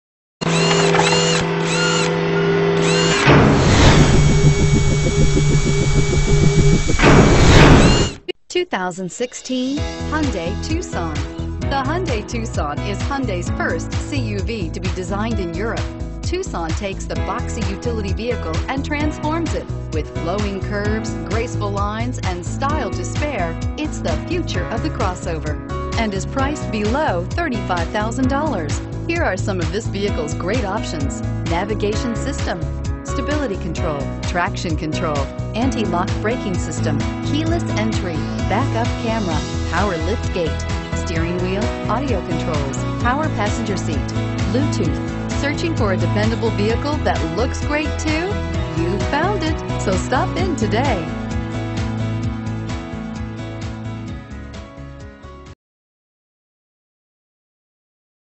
2016 Hyundai Tucson Irving TX HGU026965
2016 Hyundai Tucson Limited

Freeman Auto
1840 E Airport Fwy

Pick up this ASH BLACK 2016 Hyundai Tucson, available today at Freeman Auto. Featuring Automatic transmission, it has 0 miles. This could be the one you've been searching for.